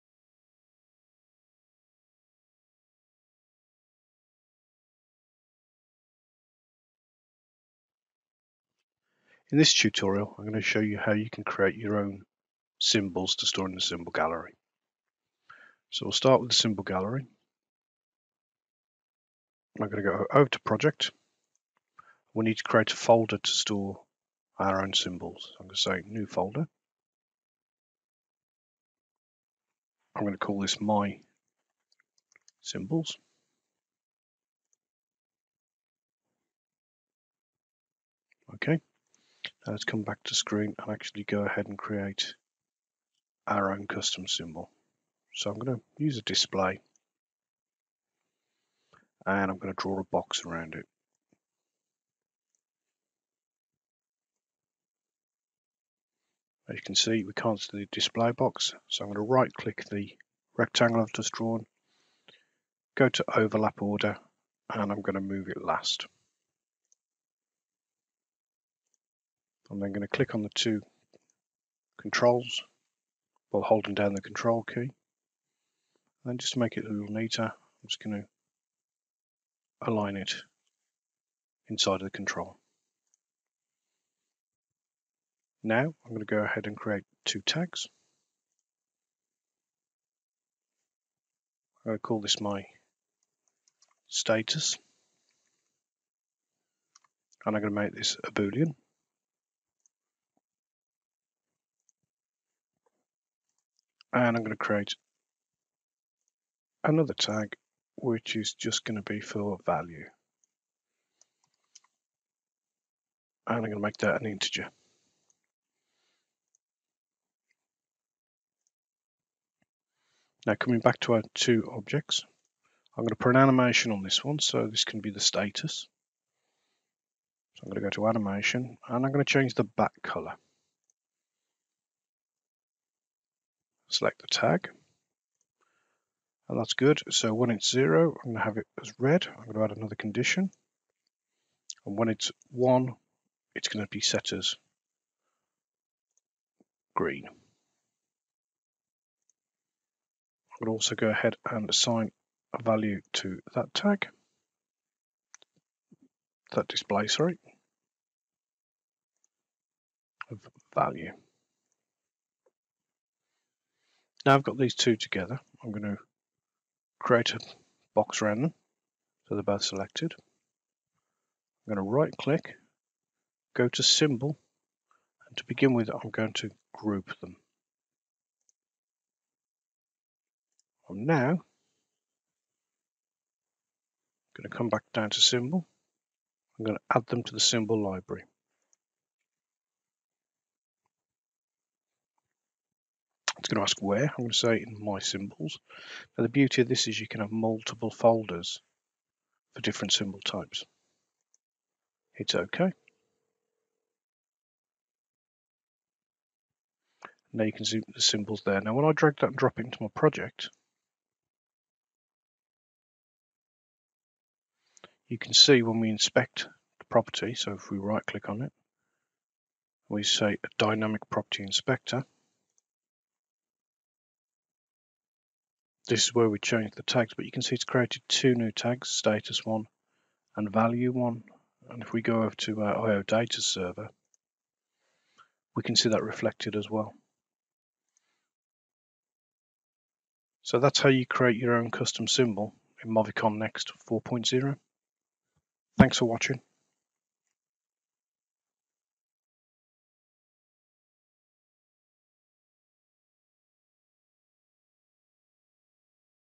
Movicon Next 4 creating custom symbols
In this video,  we show you how to create your own custom symbols that can be used multiple times in your applications.